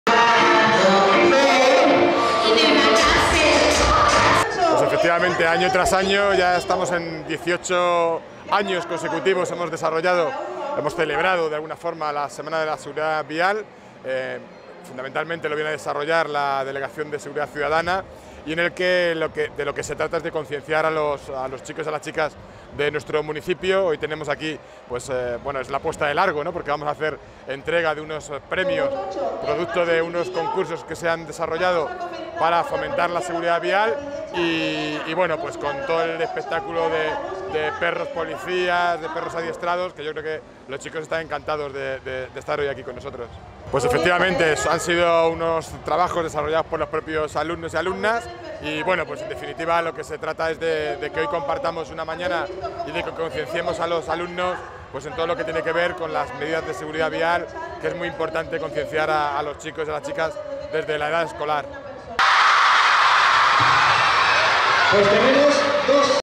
Javier Ayala "la Semana de la Seguridad Vial de Fuenlabrada cumple 18 años"
El alcalde Javier Ayala, acompañado por la concejala de Seguridad Silvia Buabent,  ha sido el encargado de entregar los premios a los ganadores del concurso de seguridad vial al que se han presentado 500 trabajos  de veinte colegios.

Ayala ha recordado que en esta edición se cumplen 18 años de la Semana de la Seguridad Vial en nuestra ciudad, una apuesta de los sucesivos gobiernos municipales por la educación a edad temprana en el respeto y conocimiento de las normas de tráfico.

El edil ha destacado la calidad y originalidad de los trabajos presentados, “la mayoría vídeos pero también algunos dibujos que muestran la enorme creatividad de nuestros escolares, así como su concienciación en torno a la importancia de la seguridad  vial “.